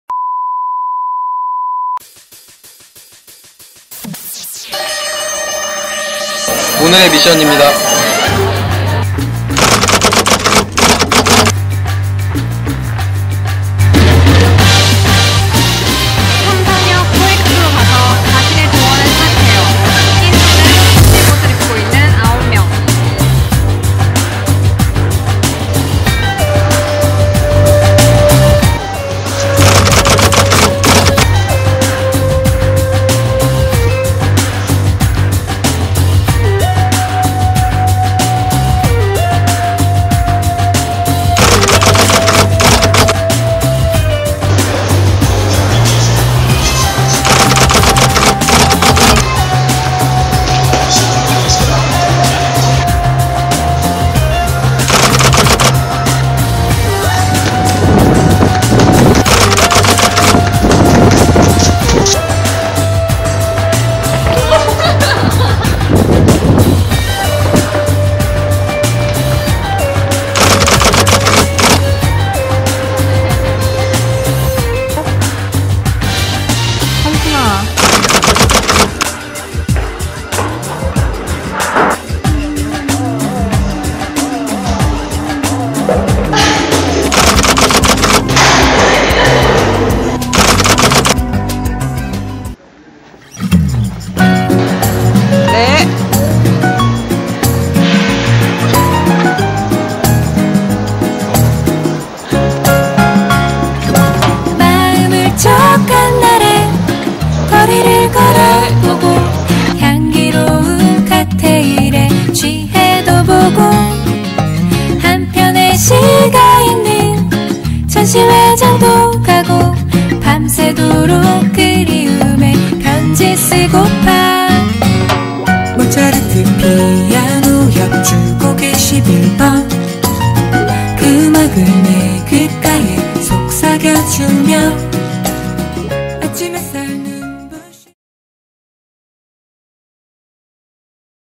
사이버 코엑스 19조 '아이코' 팀미션.
안녕하세요 사이버 코엑스 6기 19조 아이코(아이러브 코엑스)입니다!
저희 팀은 런닝맨을 패러디했습니다~ 재미있게 봐주세요:)